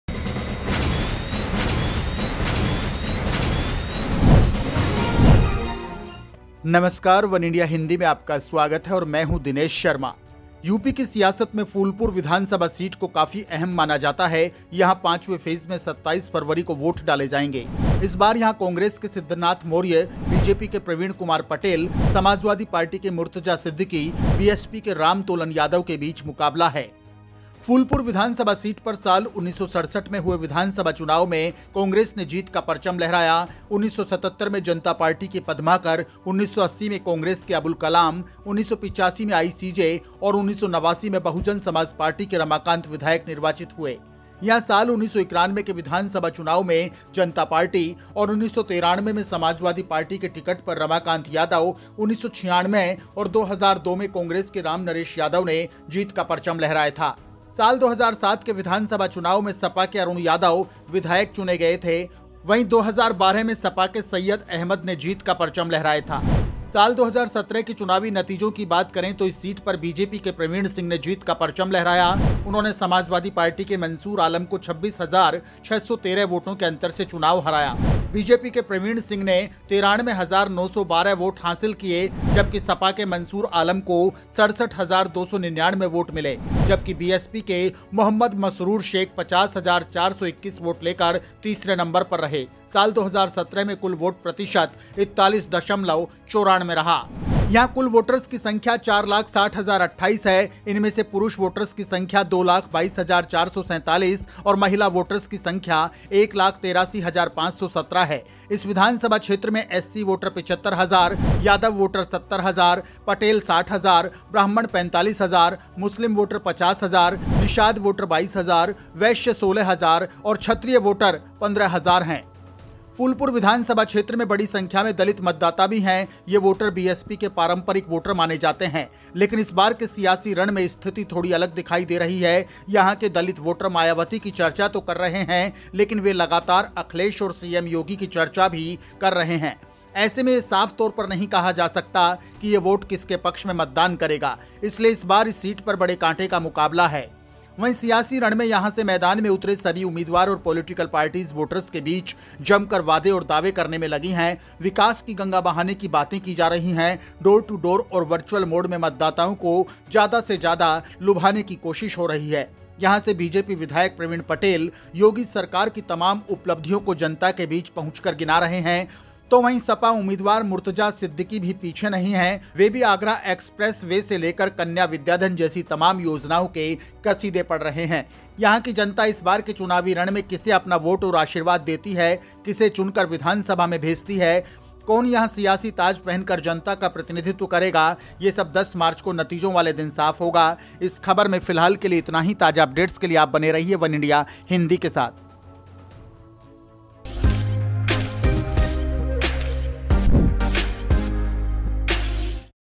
UP election 2022: Phulpur assembly seat पर इस बार क्यों है कांटे की लड़ाई ? | वनइंडिया हिंदी
Phulpur assembly seat is considered to be very important in the politics of UP. Here in the fifth phase, votes will be cast on February 27. This time here Siddhanth Maurya of Congress, Praveen Kumar Patel of BJP, Murtaza Siddiqui of Samajwadi Party, Ram Tolan Yadav of BSP There is a large number of Dalit voters in Phulpur assembly constituency. These voters are considered to be the traditional voters of BSP. But this time in the political battle, the situation seems to be getting a little different.The Dalit voters here are discussing Mayawati. But they are also constantly discussing about Akhilesh and CM Yogi. In such a situation, it cannot be said clearly in whose favor this vote will vote. There is a big thorn in the seat.

यूपी की सियासत में  फूलपुर विधानसभा सीट को काफी अहम माना जाता है.यहां पांचवे फेज में 27 फरवरी को वोट डाले जाएंगे.इस बार यहां कांग्रेस के सिद्धनाथ मौर्य, बीजेपी के प्रवीण कुमार पटेल, समाजवादी पार्टी के मुर्तजा सिद्दीकी, बीएसपी के राम तौलन यादव के बीच मुकाबला है.फूलपुर विधानसभा क्षेत्र में बड़ी संख्या में दलित मतदाता भी हैं.ये वोटर बीएसपी के पारंपरिक वोटर माने जाते हैं.लेकिन इस बार के सियासी रण में स्थिति थोड़ी अलग बनती दिखाई दे रही है.यहां के दलित वोटर मायावती की चर्चा तो कर रहे हैं.लेकिन वे लगातार अखिलेश और सीएम योगी की चर्चा भी कर रहे हैं.ऐसे में यह साफ तौर पर नहीं कहा जा सकता है कि ये वोट किसके पक्ष में मतदान करेगा.इसलिए इस बार इस सीट पर बड़े कांटे का मुकाबला है.